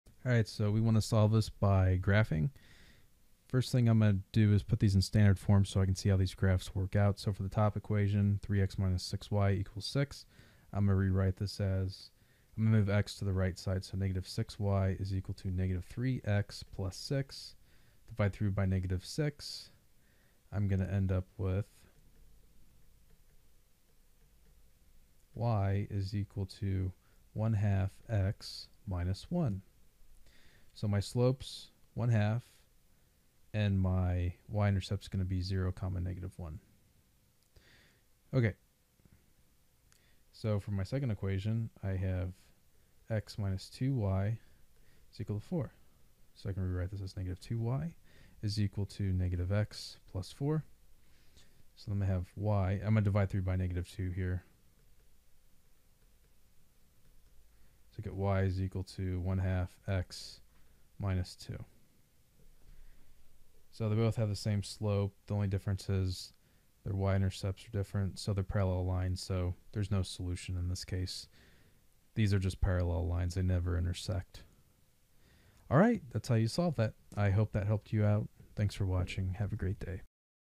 3x-6y = 6, x - 2y = 4 , Solve the following systems by graphing
3x-6y = 6, x - 2y = 4 , Solve the following systems by graphing